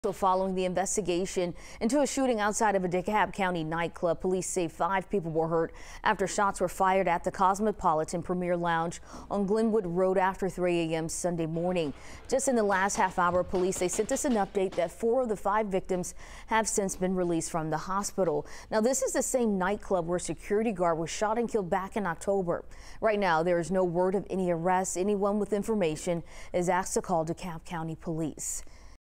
5 injured following shooting at DeKalb County nightclub
Four of the five victims have been released from the hospital.